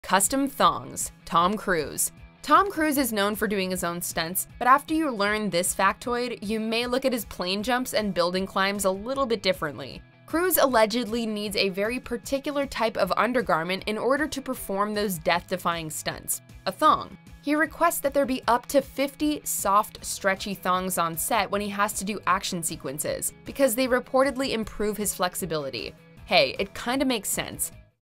Why Tom Cruise Requested 50 Thongs on Set!
For this series, we’ll be looking at the most surprising, noteworthy things actors have reportedly asked for in their contracts or on the set.

Full List